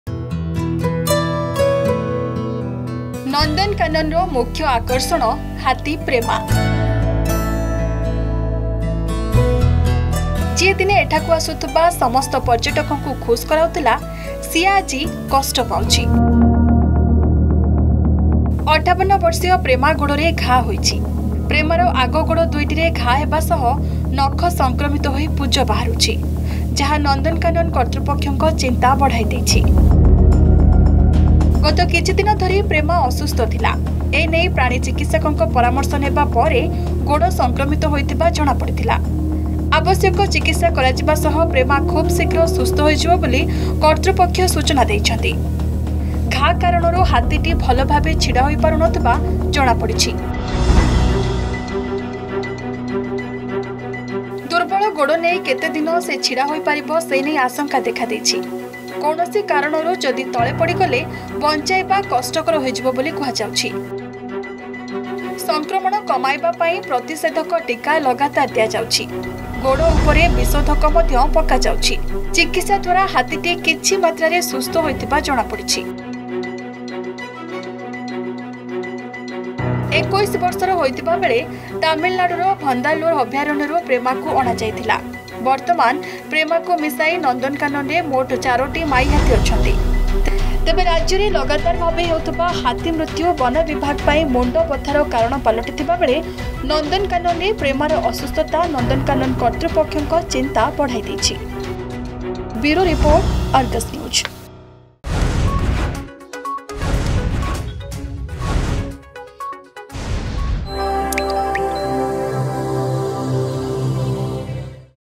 ପ୍ରେମା ଗୋଡରେ ଘା'
ପ୍ରେମା ଭଲ ହୋଇଯିବ ତ ?
୫୮ ବର୍ଷୀୟ ପ୍ରେମା ଗୋଡରେ ଘା ହେଇଛି। ପ୍ରେମାର ଆଗ ଗୋଡ଼ ଦୁଇଟିରେ ଘା ହେବା ସହ ନଖ ସଂକ୍ରମିତ ହୋଇ ପୂଜ ବାହାରୁଛି।